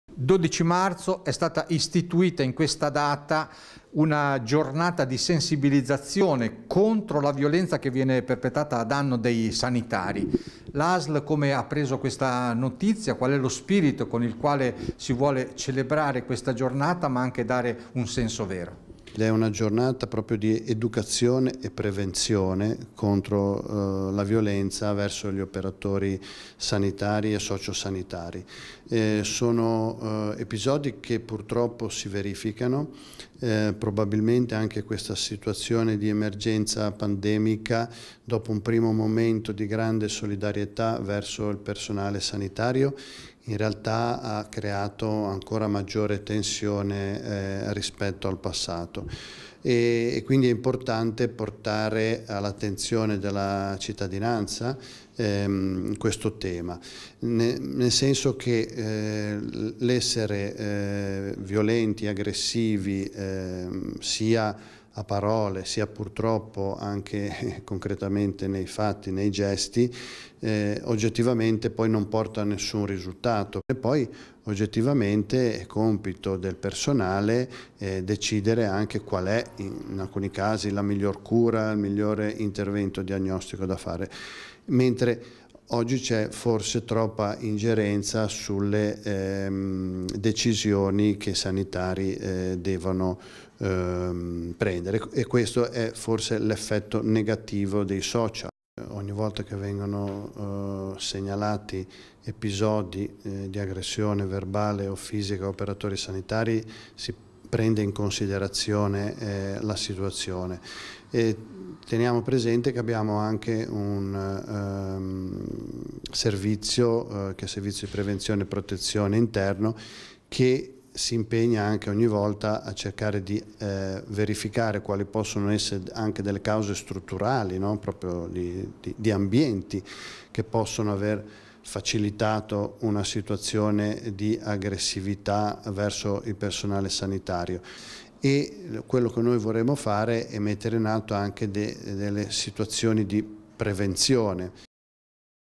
Giornata contro la violenza al personale sanitario 12-03-2022
Negli ultimi cinque anni segnalate 272 aggressioni ai danni di operatori sanitari ASL5.
E proprio durante la Giornata contro la violenza sugli operatori sanitari questa sera alle ore 21.20 andrà in onda un approfondimento.
Ascoltiamo la presentazione di Paolo Cavagnaro, direttore di Asl 5.